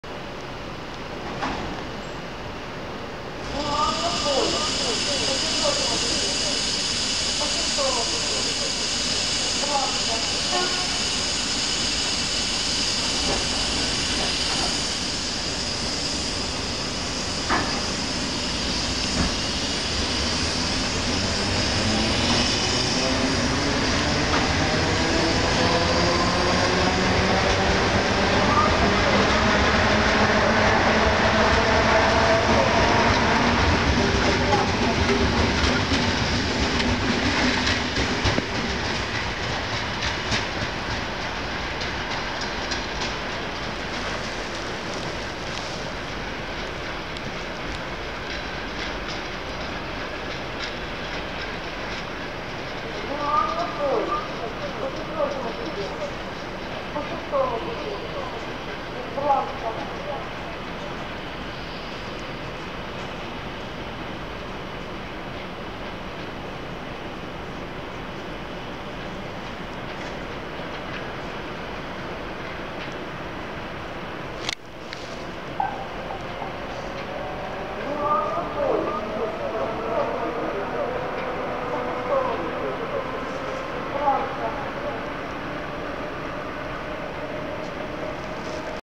Электропоезд ЭР2Т отправление станция Нара(Наро-Фоминск)28.01.2015
Электропоезд ЭР2Т рейсом Нара-Москва Киевская отправляется от 4 пути 2 платформы станции Нара, Киевское направление МЖД. г. Наро-Фоминск. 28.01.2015